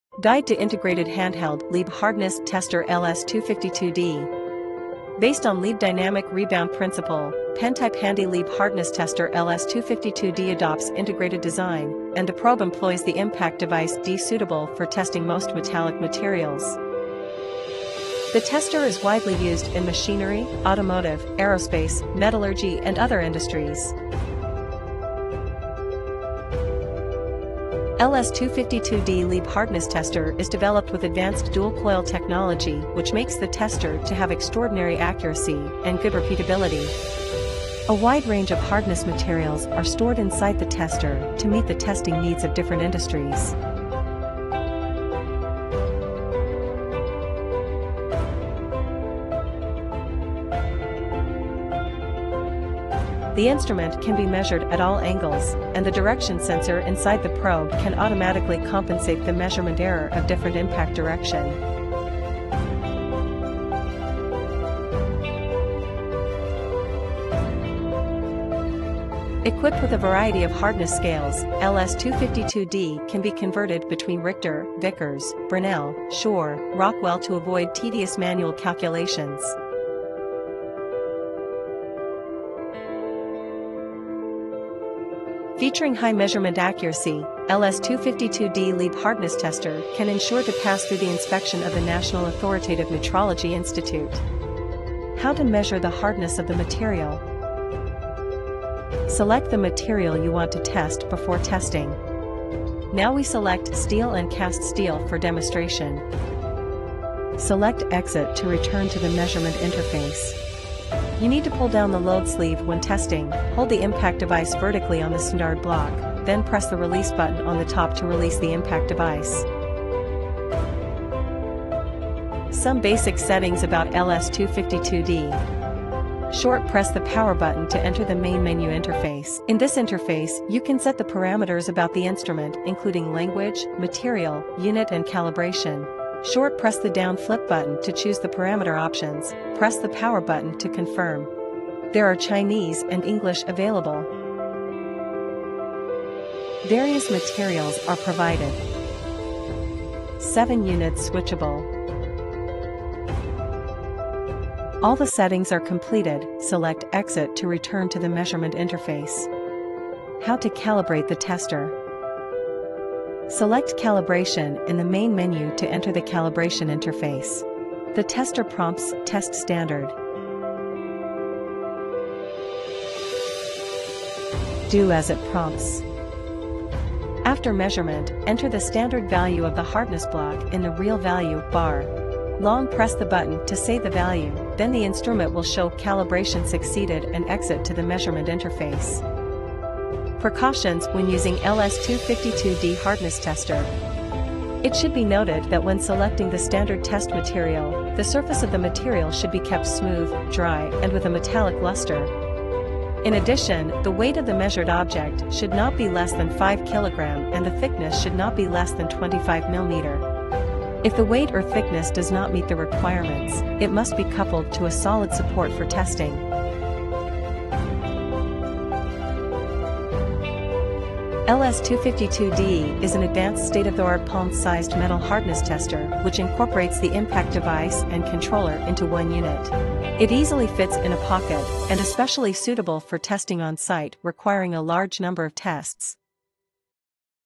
LS252D Leeb Hardness Tester Feature Operation Video
LS252D is an advanced state-of-the-art palm sized metal hardness tester which incorporates the impact device and controller into one unit. It easily fits in a pocket.  Equipped with impact device D, LS252D is especially suitable for almost any object, polished parts and heat-treated surfaces or the situation requiring a large number of tests.